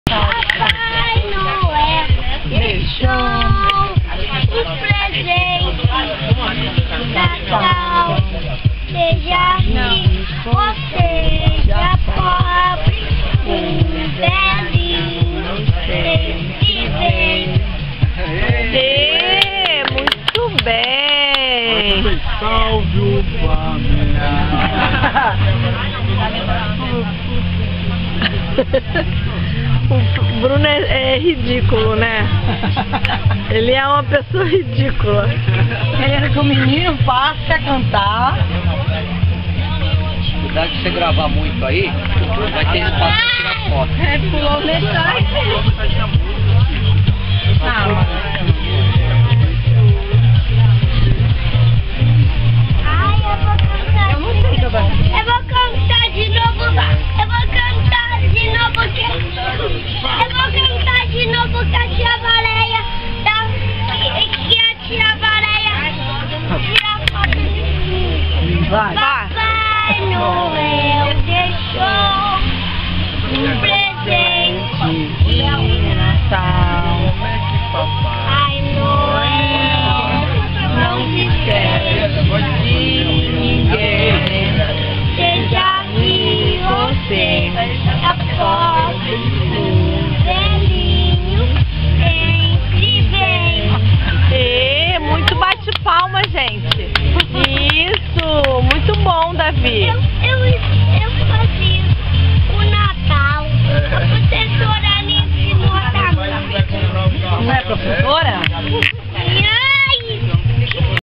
Davi desejando Feliz Natal
Esse é meu sobrinho de 4 anos, Davi !! Desejando feliz natal em plena praia das caravelas em Búzios - Rio de Janeiro.